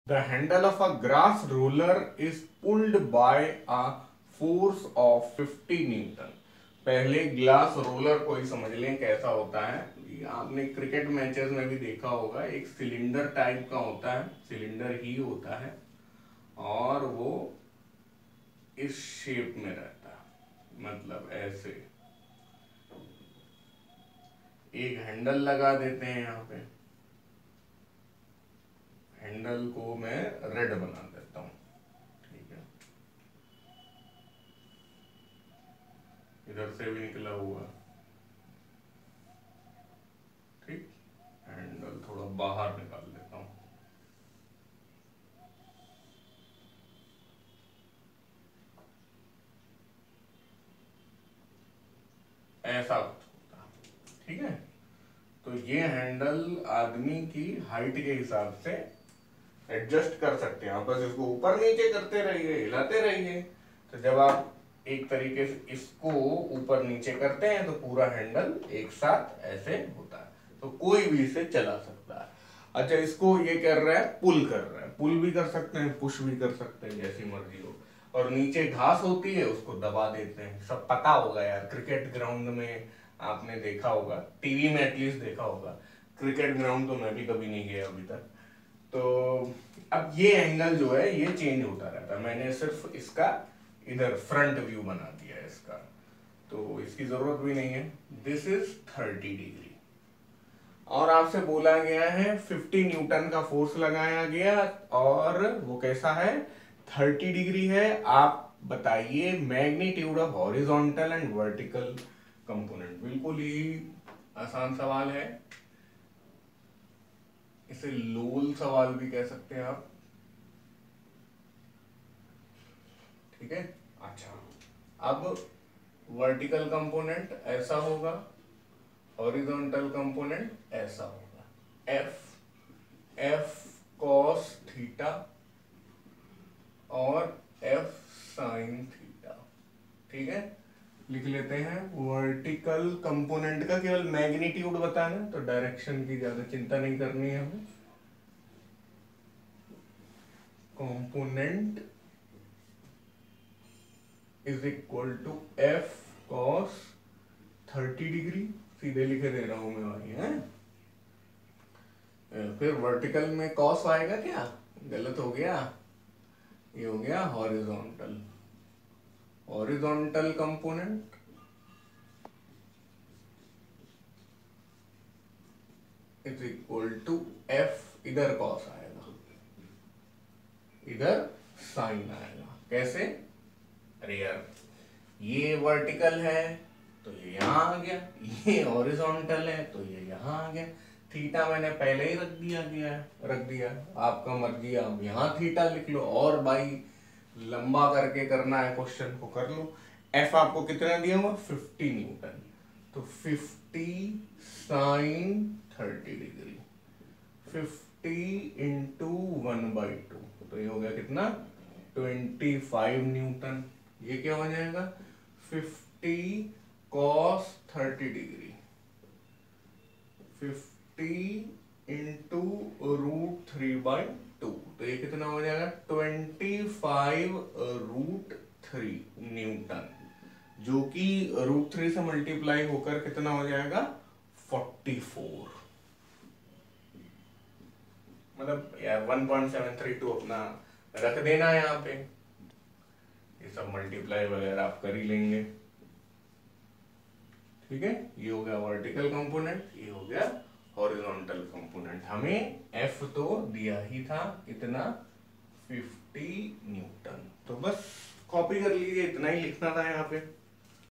Motion in a Plane Q7 Nootan Physics Numericals
The handle of a grass-roller is pulled by a force of 50 N. If the handle makes an angle of 30° with the
horizontal, calculate the magnitudes of the horizontal and the vertical components of the force by. a vector diagram.